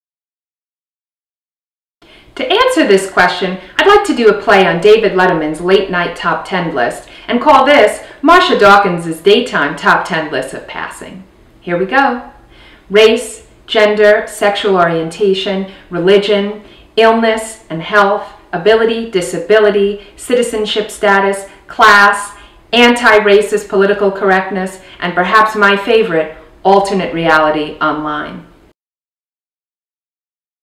Clearly Invisible FAQ - Dr. Dawkins's Top 10 List of Passing
Dr. Dawkins responds to readers' FAQ about how people are passing today.